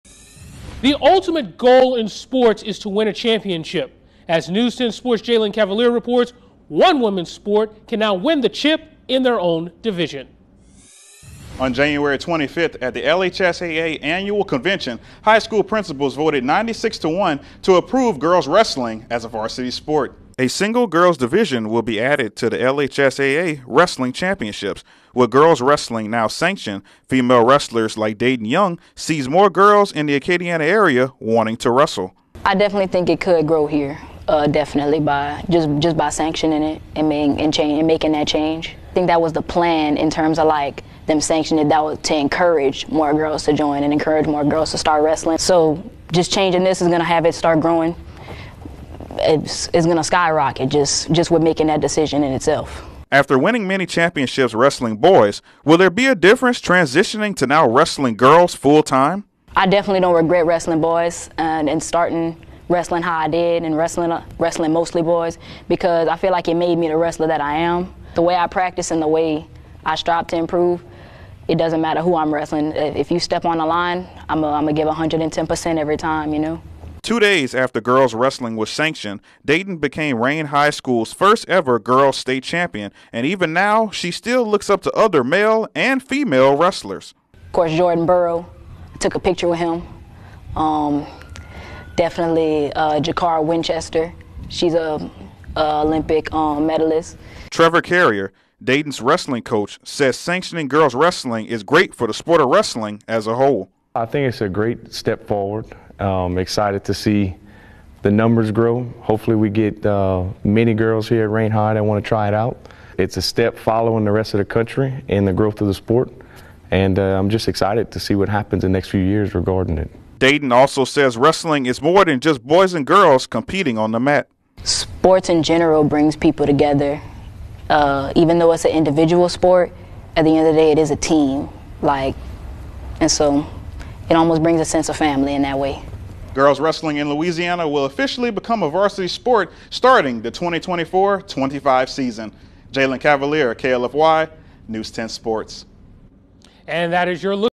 Rayne High School wrestler says LHSAA new varsity sport could bring more girls to the mat
The ultimate goal in sports is to win a championship, and one woman's sport can now win the chip in their own division.